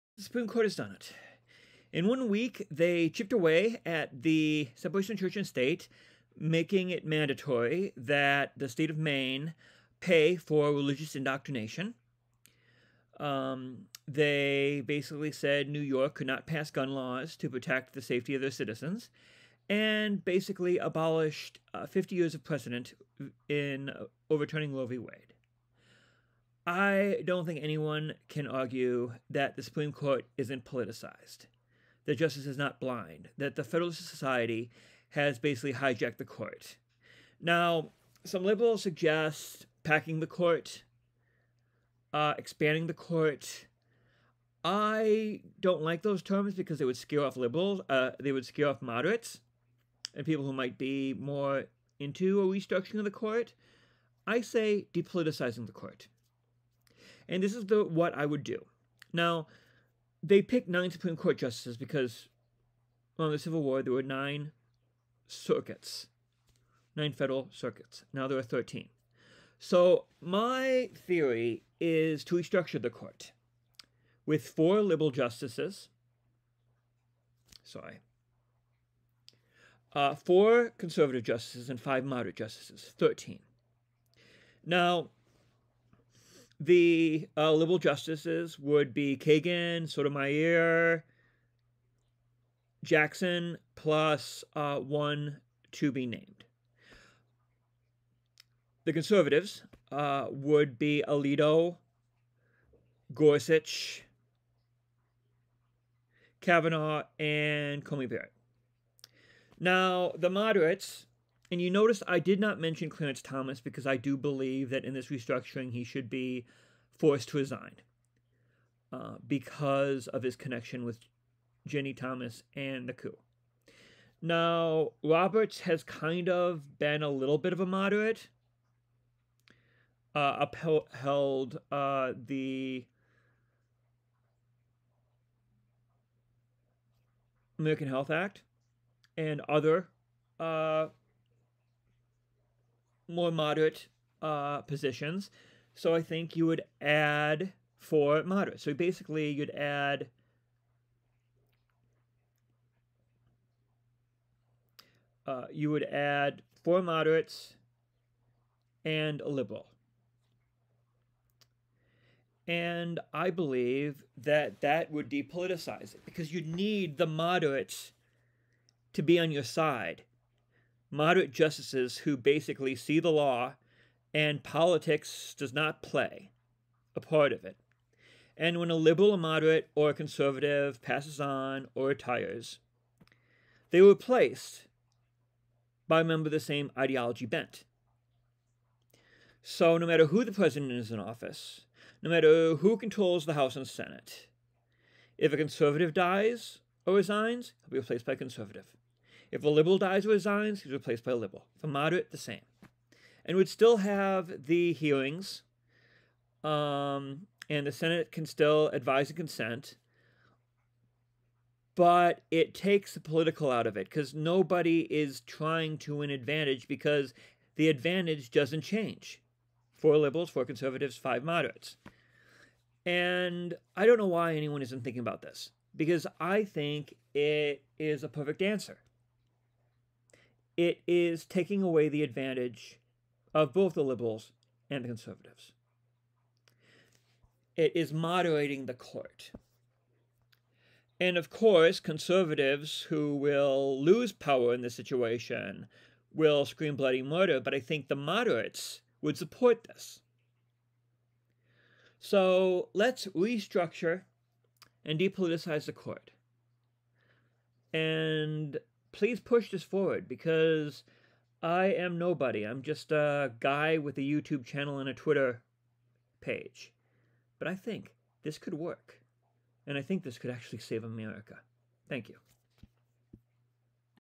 Depoliticize the Supreme Court Now
I have a proposal. the Supreme Court Is not an independent body of justice anymore. it has been hijacked by the Federalist society. Raise the justices from 9 to 13 and give no ideology a political edge